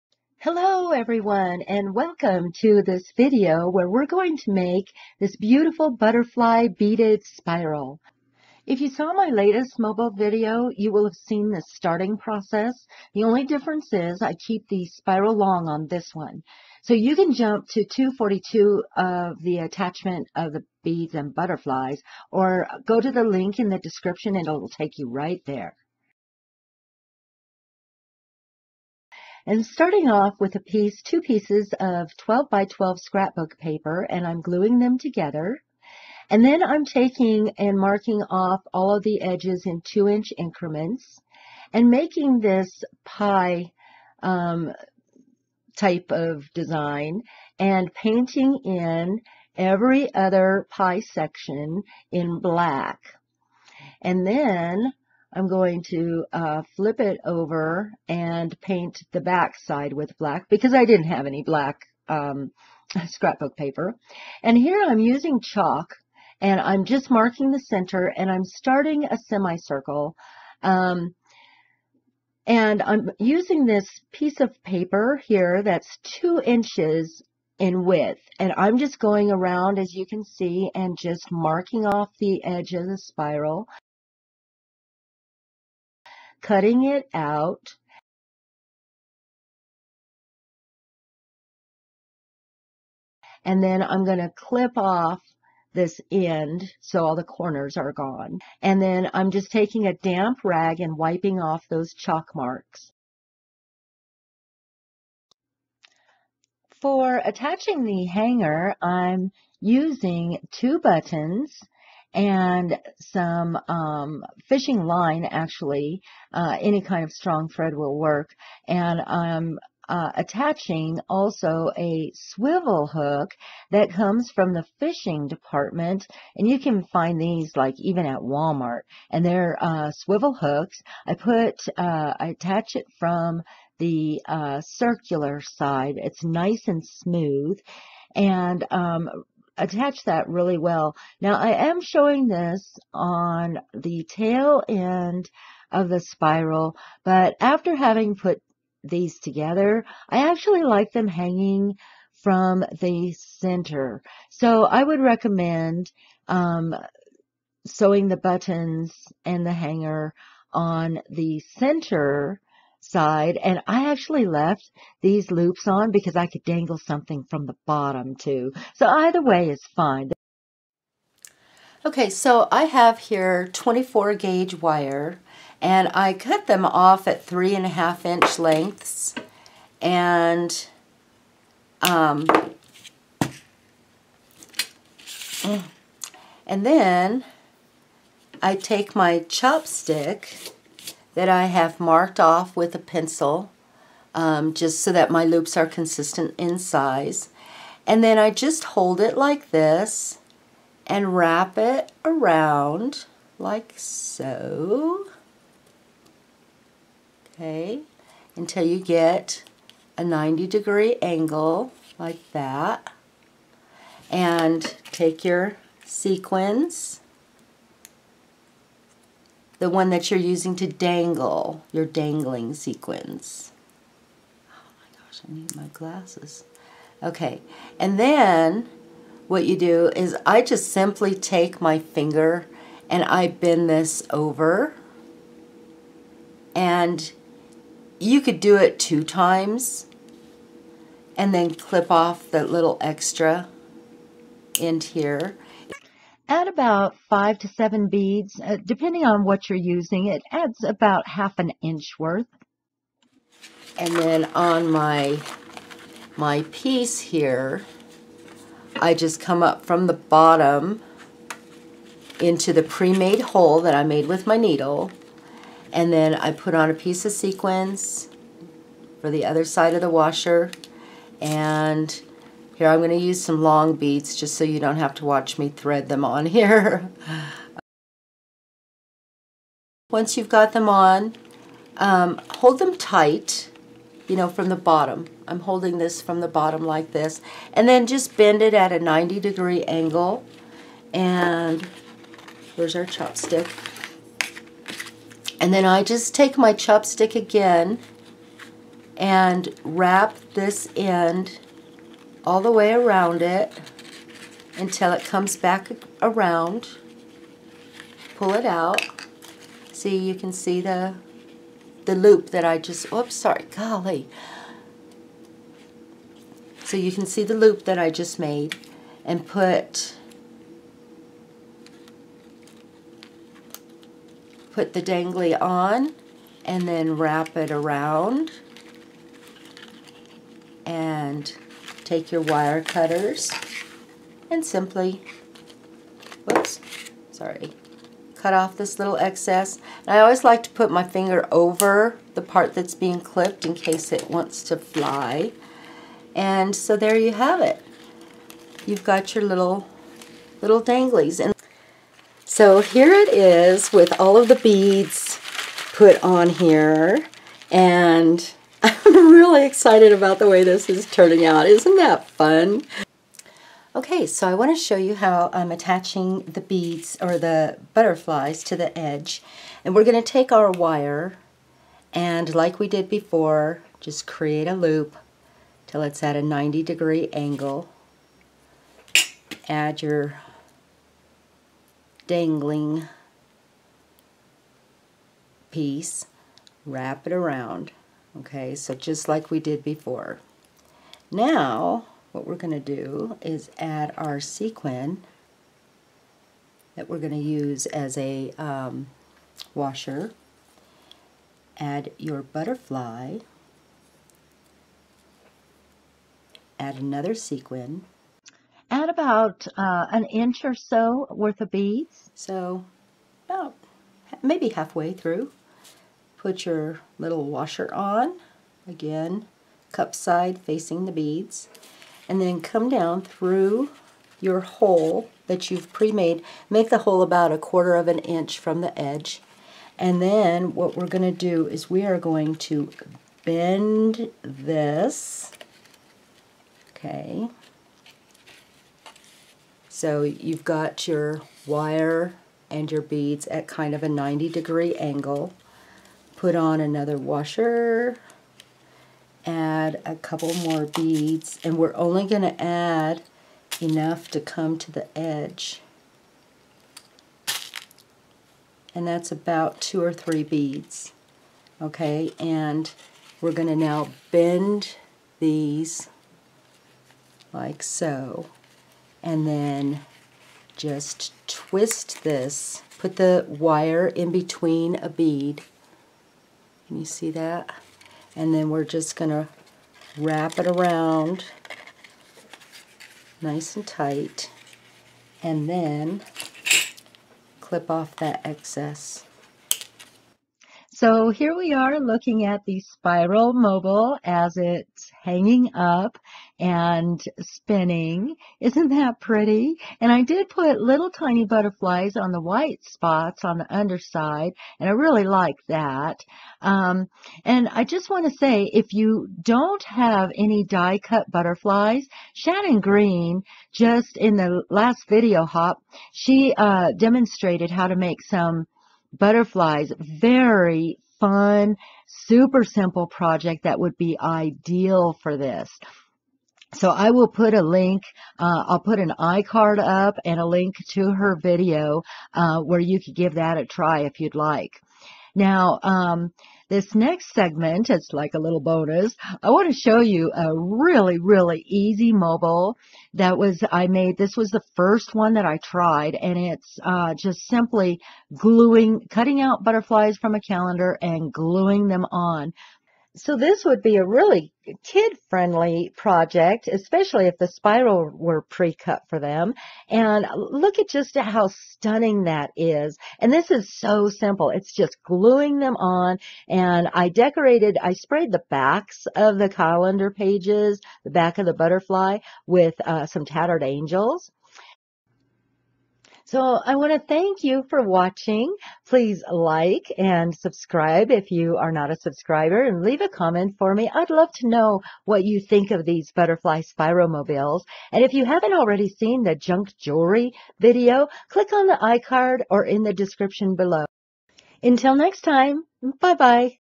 Beaded Butterfly Spiral Mobile and Easy Glued Image Mobile
There are two easy projects shown in this video. The Beaded spiral is labor-intensive but well worth the effort for the stunning results. The Glued image mobile is a kid-friendly craft. If you saw the last mobile video you can skip to the beading portion at 2:50.

If you want a really FUN, EASY and QUICK way to make pretty butterflies check out Shannon Green's Folded Butterflies video

I thought it would be fun to show a variety of projects using the Spiral Mobile foundation. There are so many possibilities.

This beautiful DIY Mobile will make a wonderful gift and a lovely addition to any interior home decor. It was made using the following materials:

2 pieces of 12x12 scrapbook paper
Americana Craft Paint - Black
Chalk
2 buttons
Berkley Fireline (this is the thread i used for sewing on the buttons and hanger)
Eagle Claw Barrel Swivel with Safety Snap Size 5
24 gauge wire
Wire Cutters
Chopstick (or any rounding tool, preferably tapered at the end)
Small scrap beads in a variety of shapes and sizes
Tim Holtz Butterfly Duo
Cheery Lynn Designs: Exotic Butterfly Medium #3 (this was the filigree / lacy one)
Spellbinders Shapeabilities Nested Butterfly Trio (the small one was used on the underside)
Butterfly images fussy cut from a calendar
Alene's tacky glue
Mod Podge (for gluing the two pieces of scrapbook paper together)
Tattered Angels Glimmer Mist (for the back of the calendar pages)